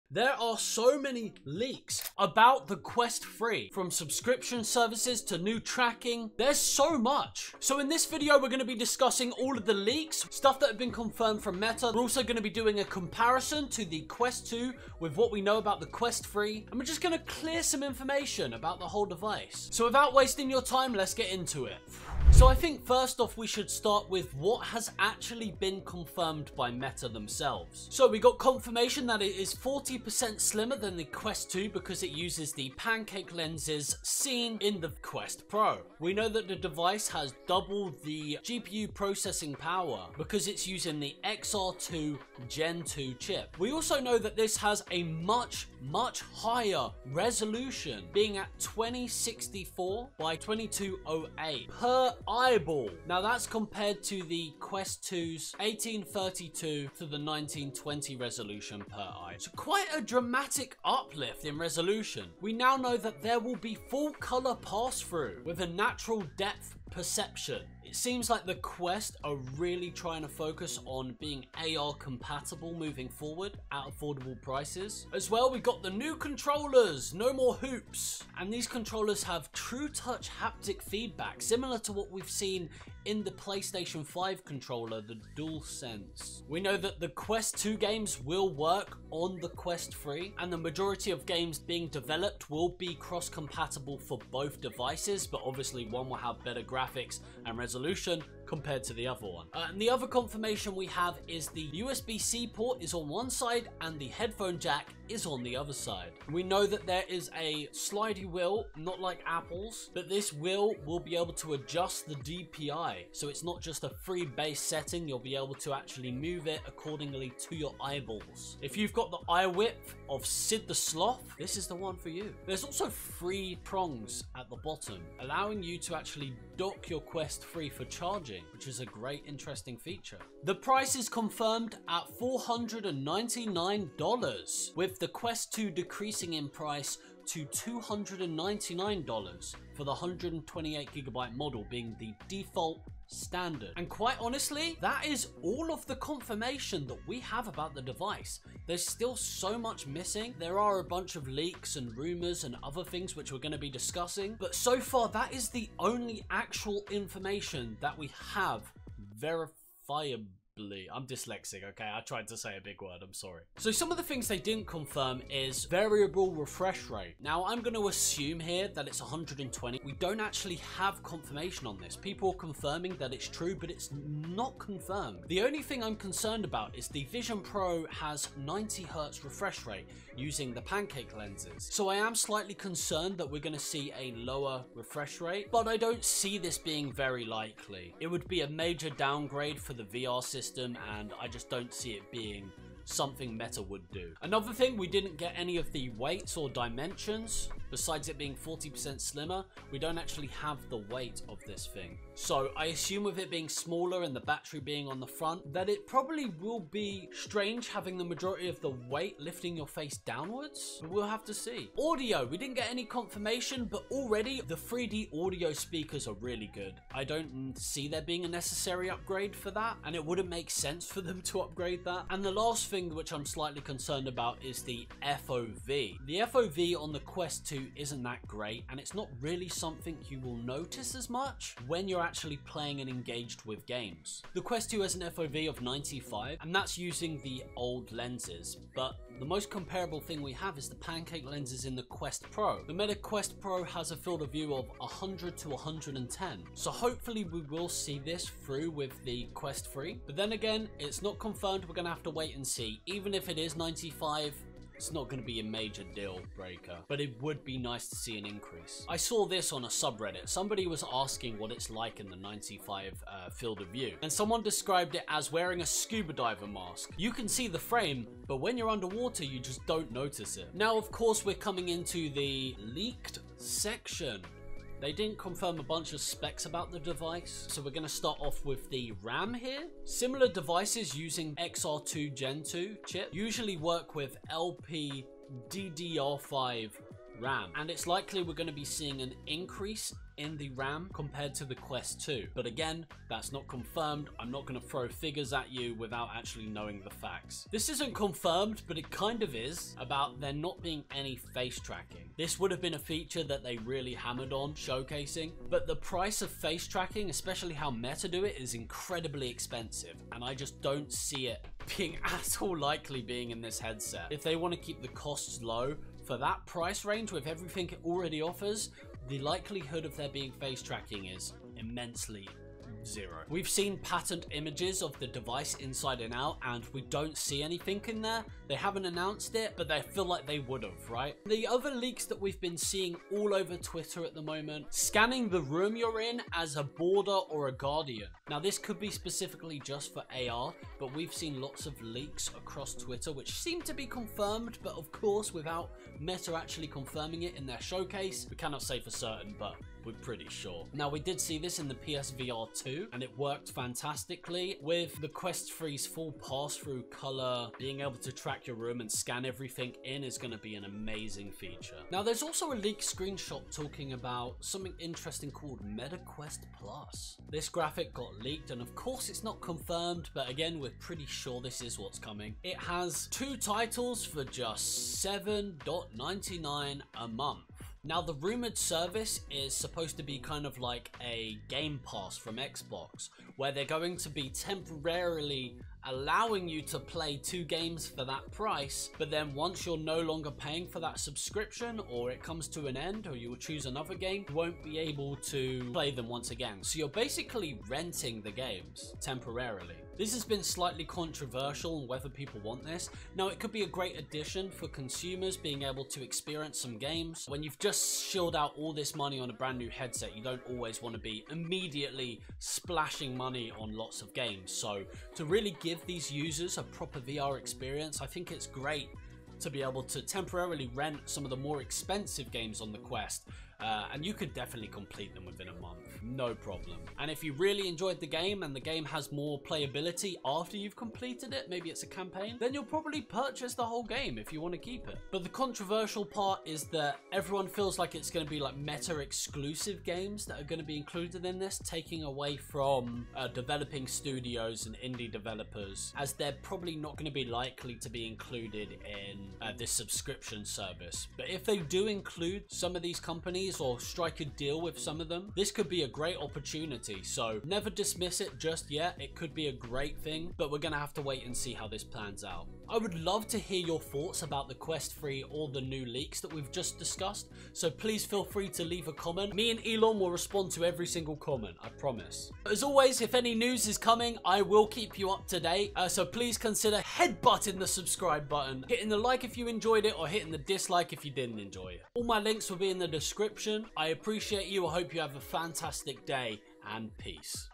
The TRUTH about the META QUEST 3
The META QUEST 3 Just Got EVEN BETTER!

Who is Lordsi?
In This Video we really deep dive into all the Meta Quest 3 Leaks and confirmed new infomation about VR & AR. Meta have annouced a bunch of new features for all quests and how they will compete with apple vision pro.  The META QUEST 3 Just Got EVEN BETTER!, We discuss everything to new spec leaks with the meta quest 3 to software. We discusses the new controllers and there haptic Trutouch haptic Feedback, dpi wheel, Charging dock, quest 2 accessories and sooo much more.

0:00 - Intro
0:27 - 40% Slimmer
0:35 - Double GPU Power - XR2 Gen 2 Chip
0:43 - Higher Resolution
1:02 - Full Color Passthrough & True Depth Sensors
1:15 - New Controllers / TruTouch haptic triggers
1:26 - Backwards Compatible Quest 2 Games
1:41 - USB C & Headphone Jack
1:47 - IPD Slider Wheel
2:02 - Dock Charging
2:10 - Price & Storage
2:45 - Refresh Rate
3:14 - Weight & Dimensions
3:31 - Audio / Speakers
3:42 - FOV
4:37 - Ram
4:57 - Face Tracking
5:34 - Room Scanning Leaks
6:01 - Meta Quest Plus Subscriptions
7:56 - Outro